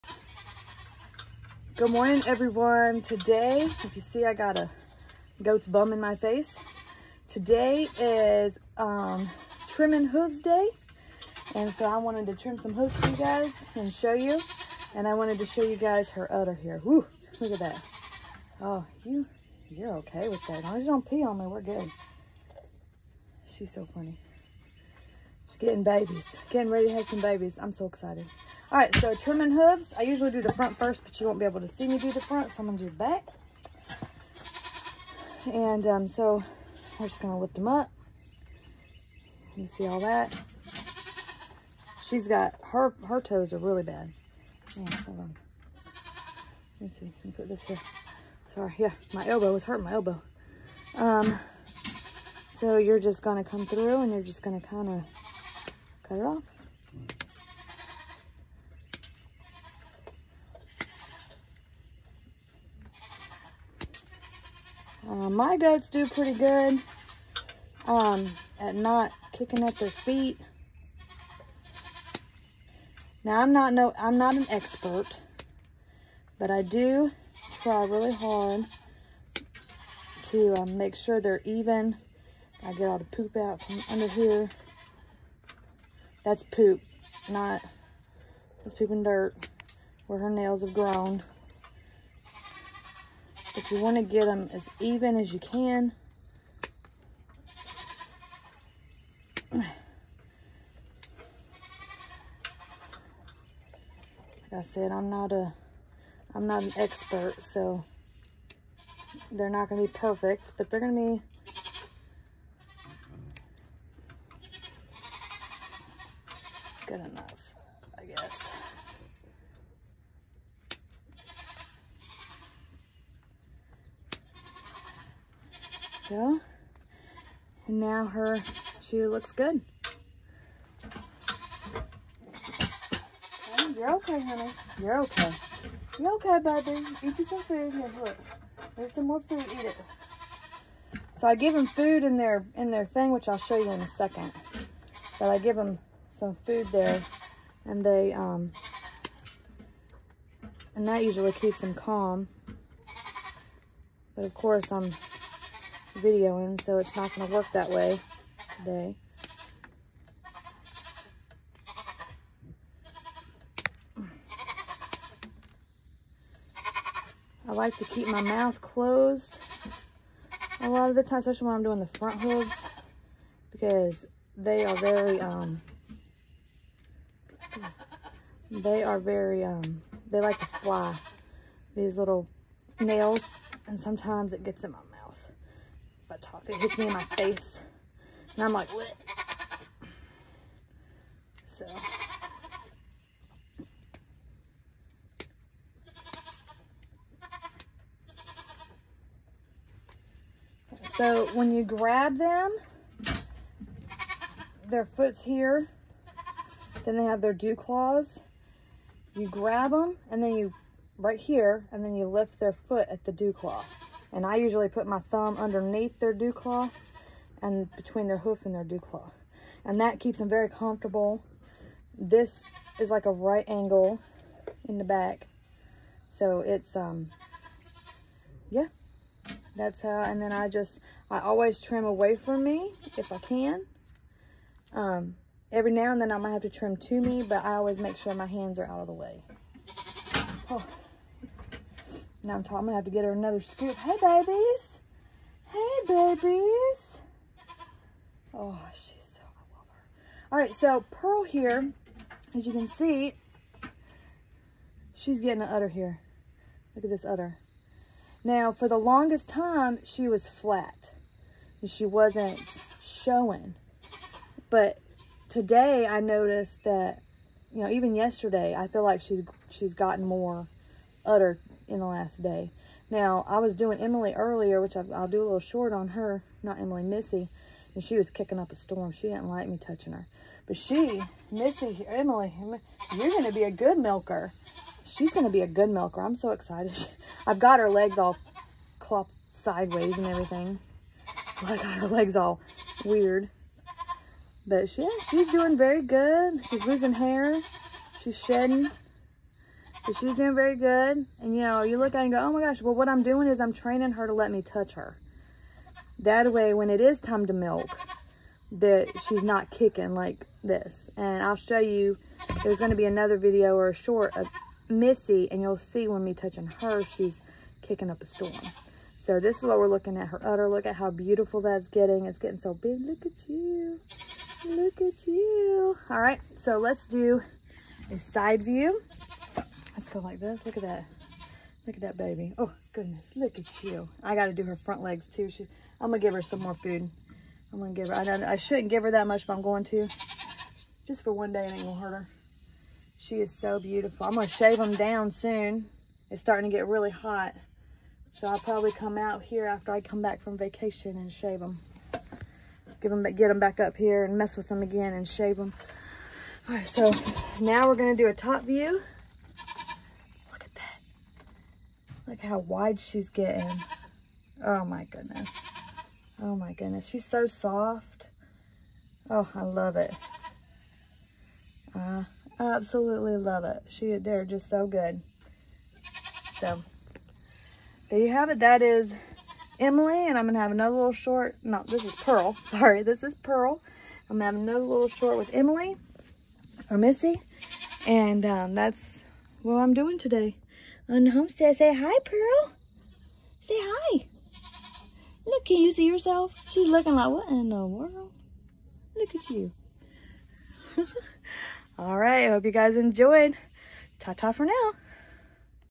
Trimming hooves
Come join me for a hooves trimming  and to see pearls growing baby bump! And her utter! She is due at the end of May! ￼￼